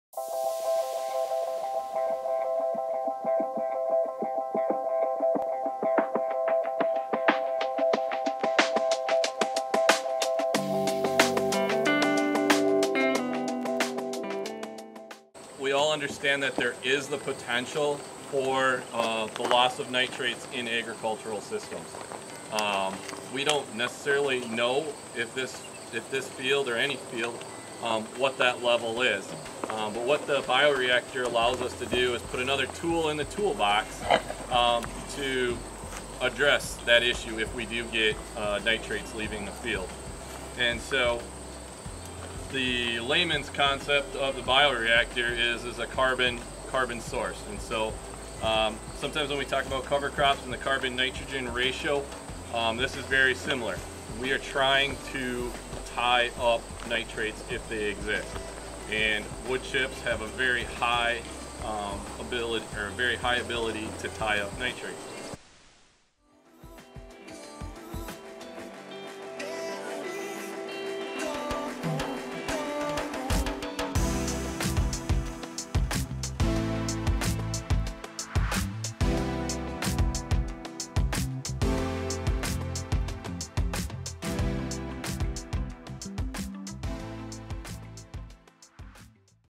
Tile Line Bark Bed Filter
Kinnard Farms hosted a tour featuring their new tile line bark bed filter! We had more than 40 people out to learn about this new practice. The filter strip is a tool that will help address the nitrogen leaving the field by utilizing the wood chips natural ability to tie up nitrates. For Kinnard Farms, they're eager to try new practices that will help reduce nutrient loss on their farm. As part of the DK Demo Farms project, they'll be working with the U.S. Geological Survey (USGS) to monitor the effectiveness of this new system. Check out this video to learn more!